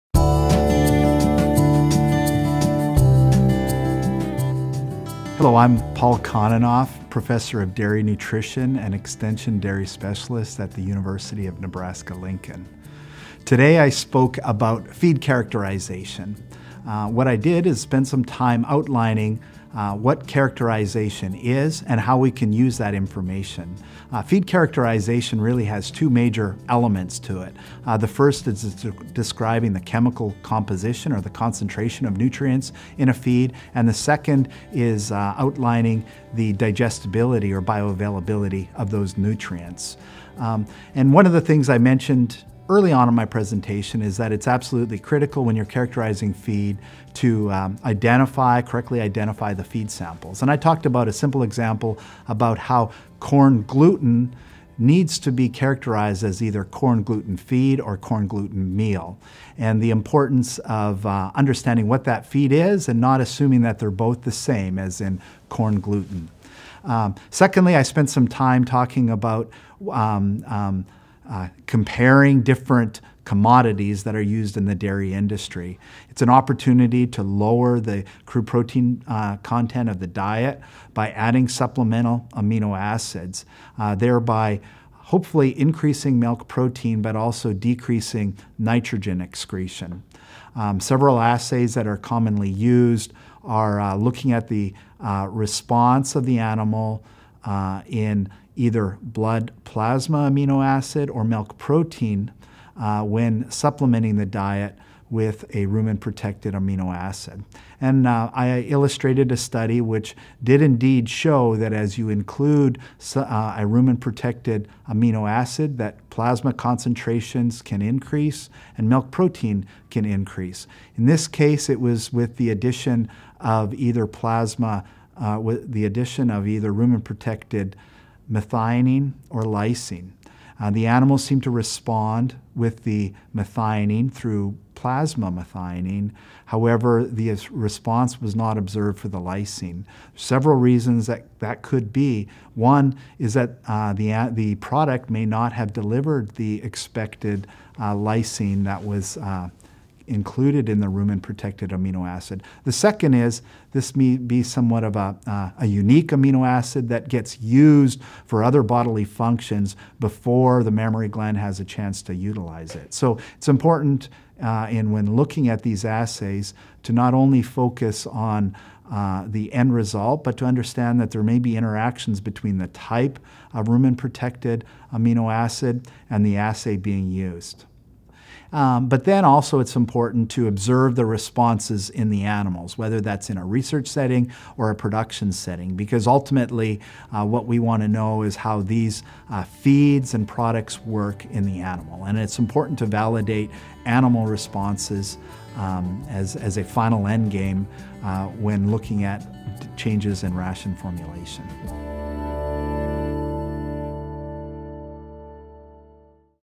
Testing feed samples and supplements for rumen and intestinal digestibility
Paul Kononoff, PhD, Professor, Department of Animal Science, University of Nebraska - Lincoln